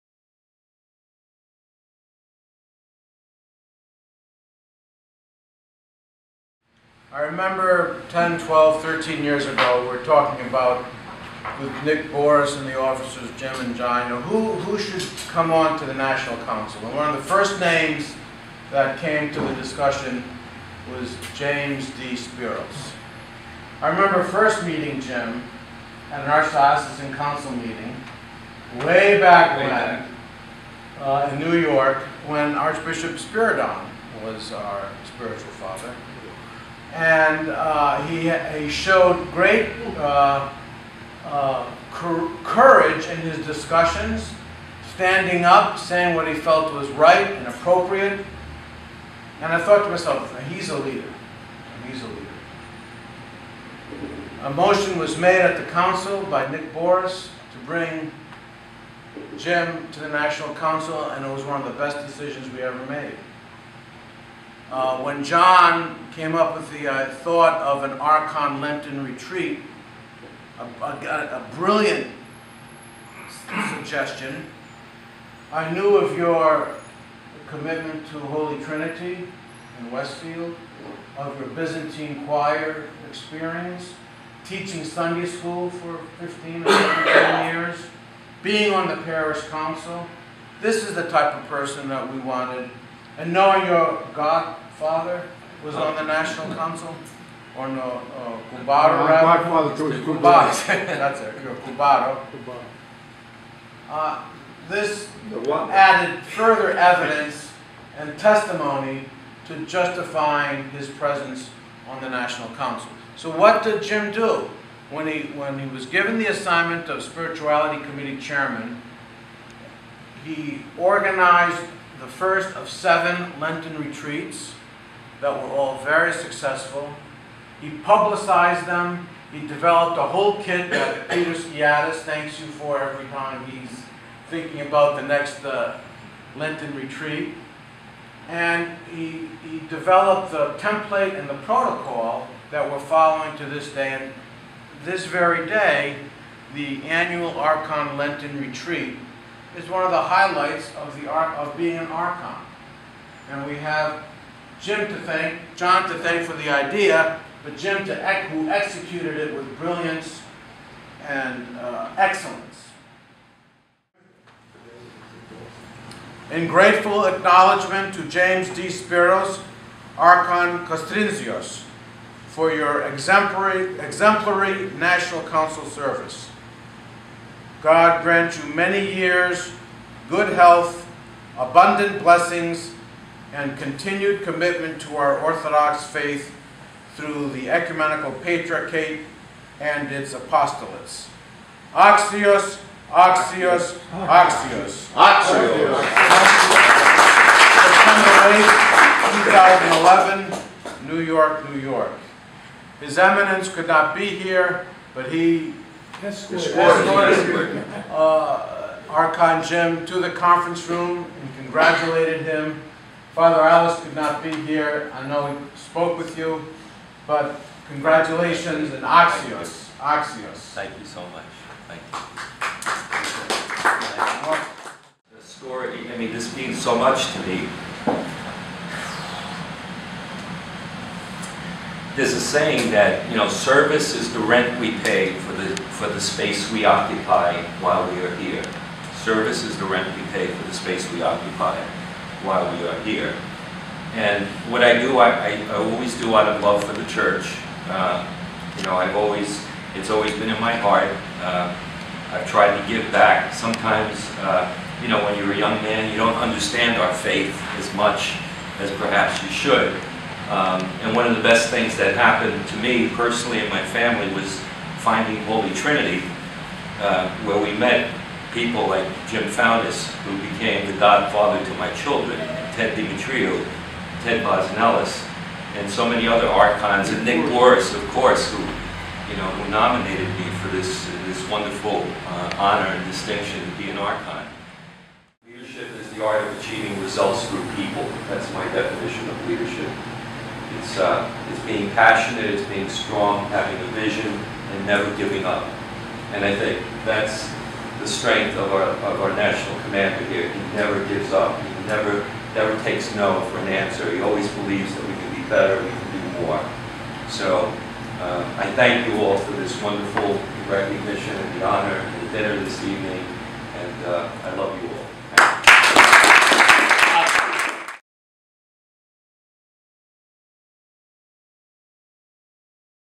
National Council of the Order of Saint Andrew honors Archon James D. Speros
On Thursday, September 8, 2011, the National Council of the Order of Saint Andrew honored Archon James D. Speros, who retired from the council. National Commander Dr. Anthony J. Limberakis, and members of the Executive Committee, presented Archon Speros with a commemorative plaque at the conclusion of their meeting, which read, "In grateful acknowledgment to James D. Speros, Archon Kastrinsios for your exemplary National Council service. God grant you many years, good health, abundant blessings and continued commitment to our Orthodox Faith through the Ecumenical Patriarchate and its Apostolates. Axios! Axios! Axios!" (Video by J. Mindala)